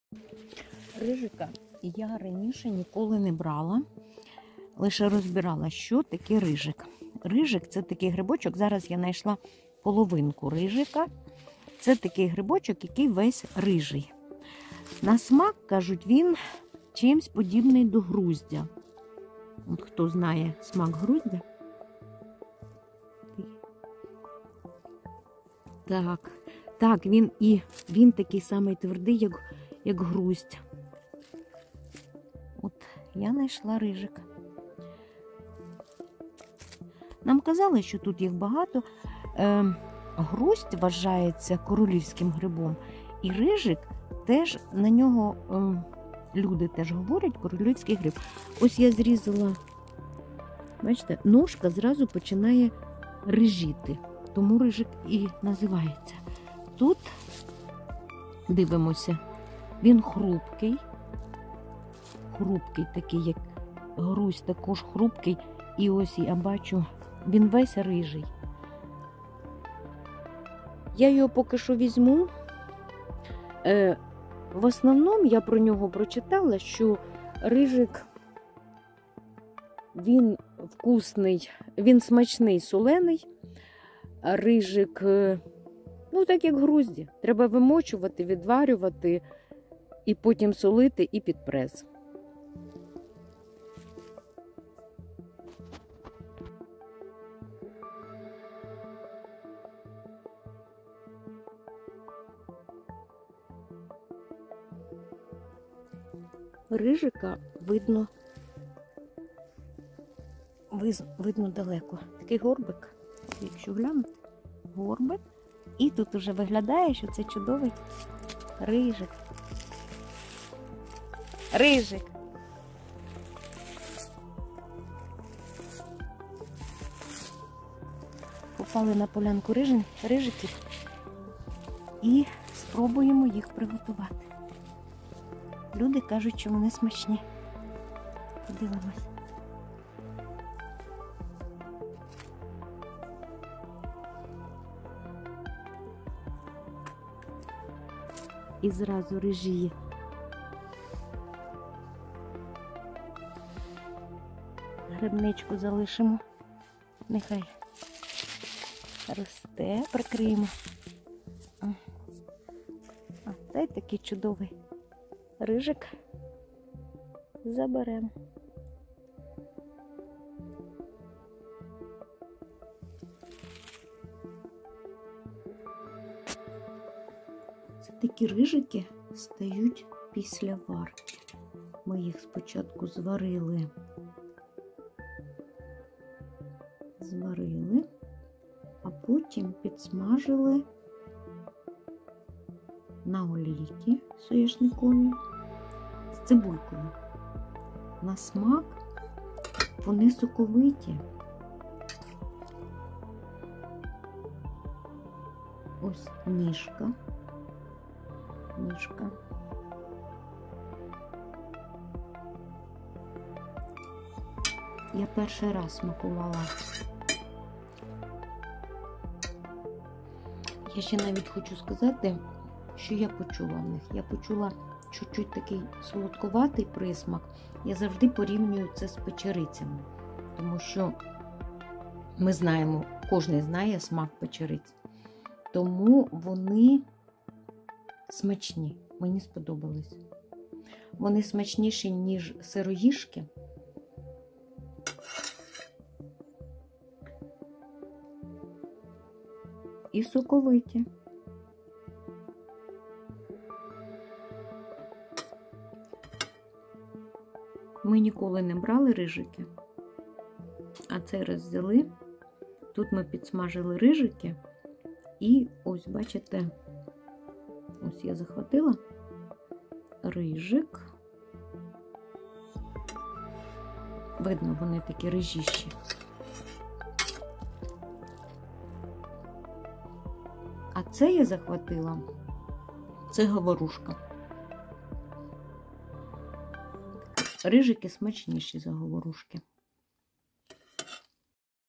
Рижики. Ми назбирали 2 корзини.
Рижики смачнi i ix в лici зараз чималенько.